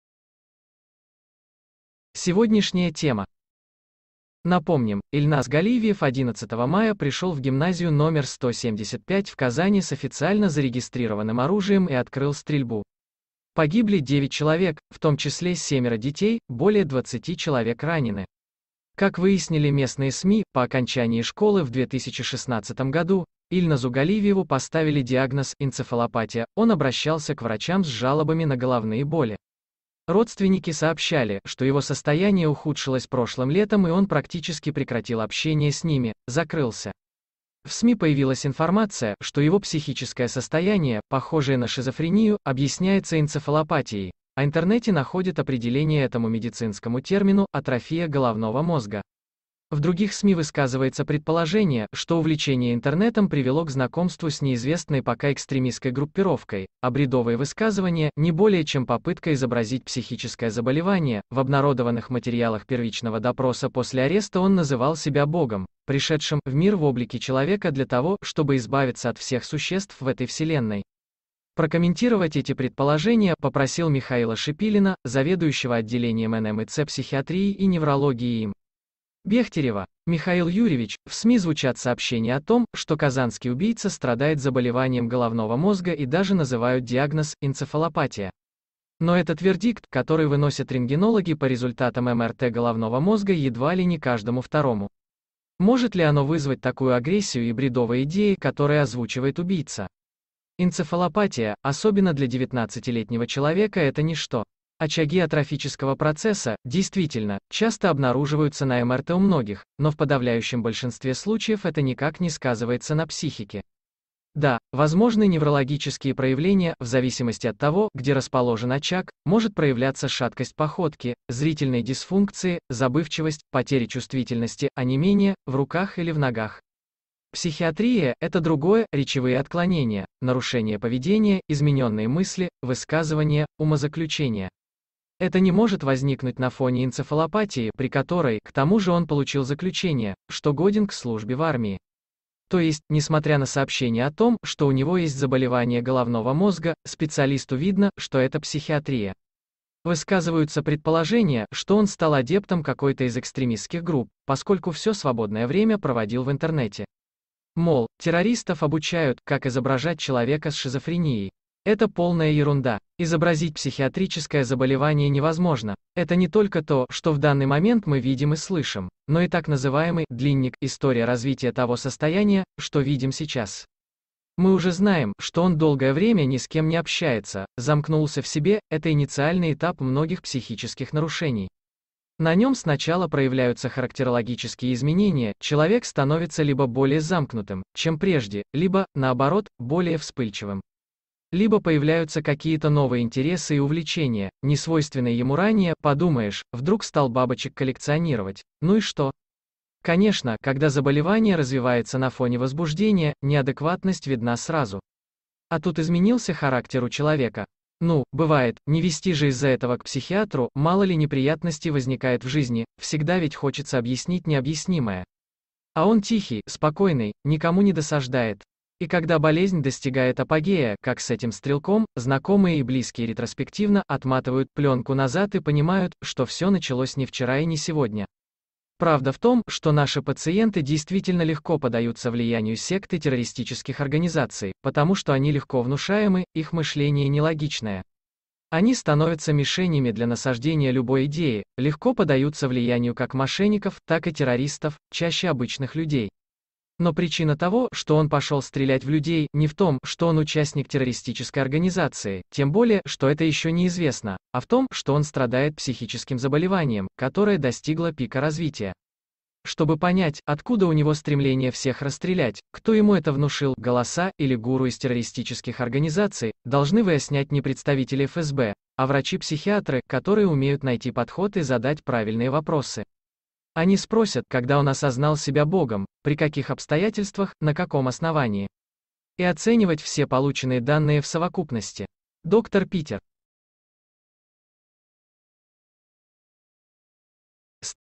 Заболевания мозга нет, а если и есть, оно ни при чем
Чтобы выяснить, что заставило 19-летнего Ильназа Галявиева расстрелять детей и учителей родной гимназии, его привезут в Москву. Как сообщают СМИ, его ждет комплексная психолого-психиатрическая экспертиза в Институте имени Сербского. До этого сообщалось, что состояние убийцы обусловлено поставленным три года назад диагнозом «энцефалопатия».